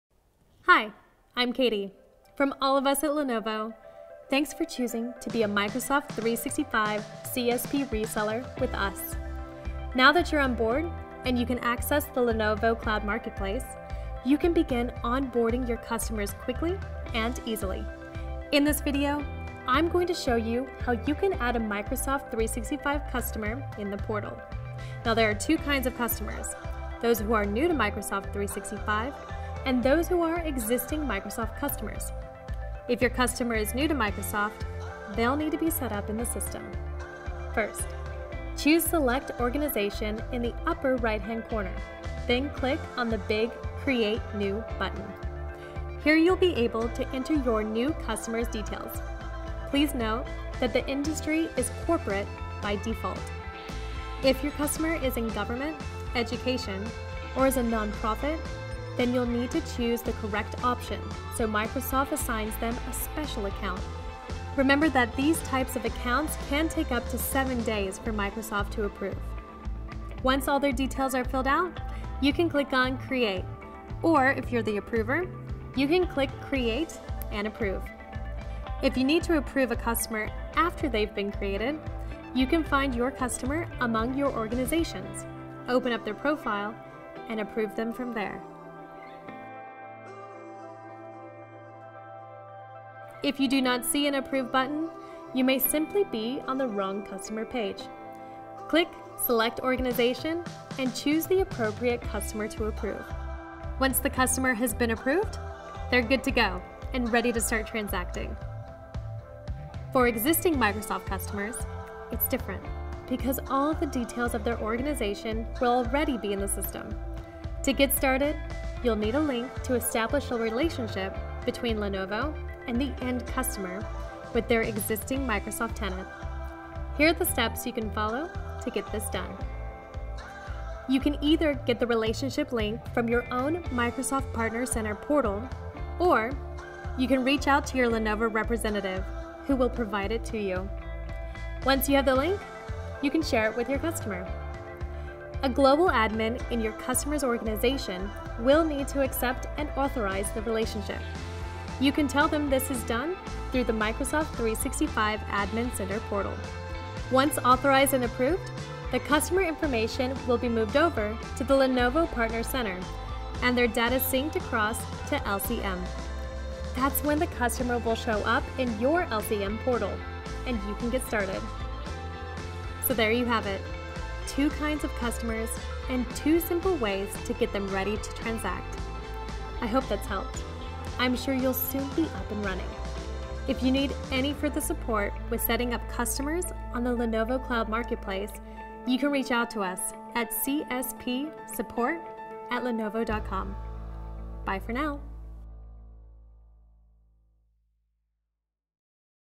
Lenovo Cloud Marketplace: Adding a Customer
The Lenovo Cloud Marketplace enables our CSP resellers to quickly generate new recurring revenue streams through a robust cloud catalog. This video walks through how to add customers in the platform.